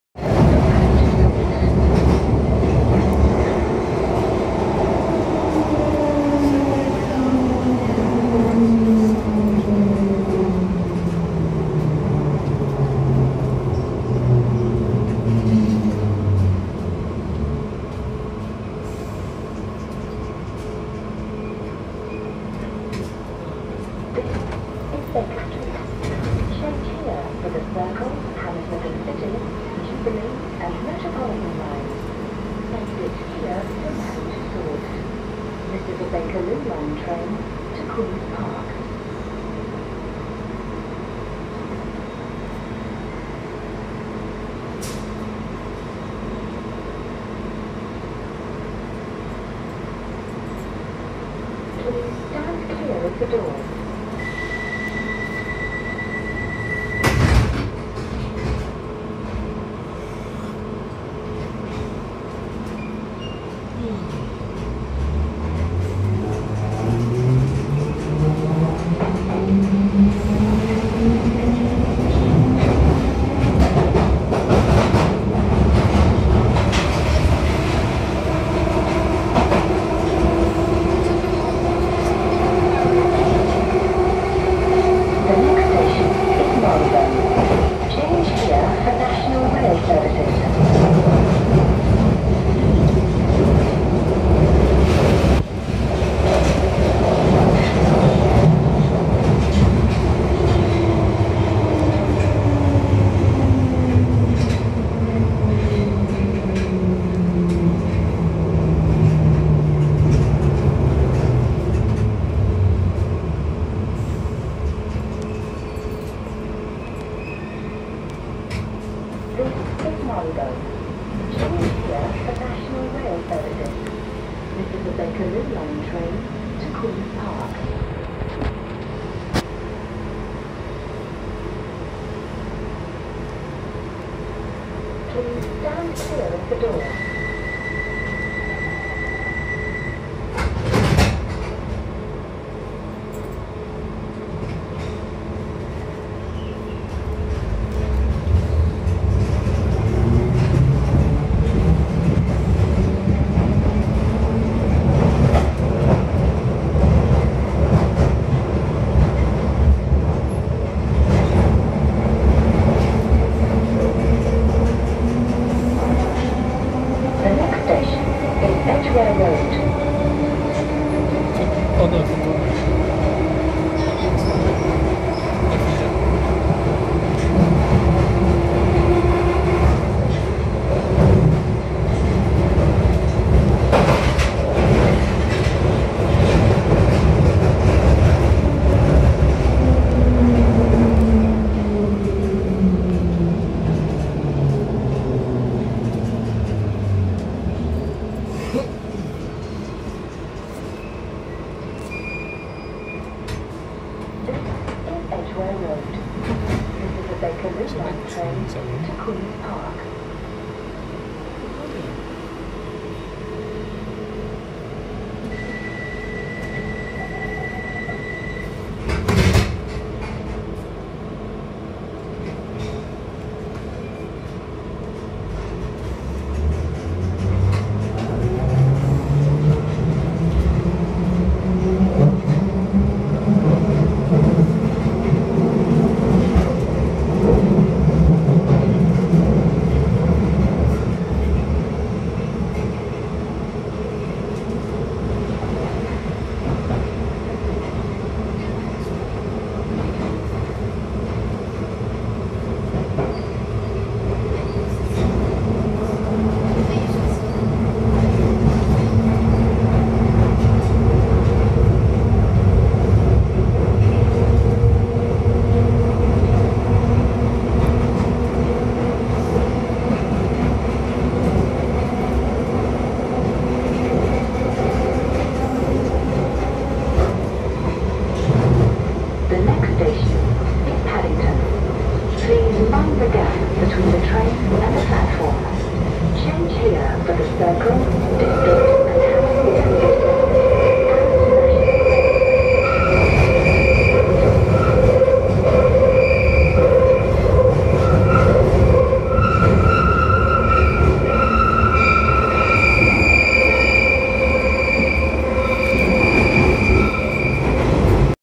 The Bakerloo Line Underground Tube, London, September 2019
On-broad the Bakerloo Line from Marylebone to Paddington Station, London, 21 September 2019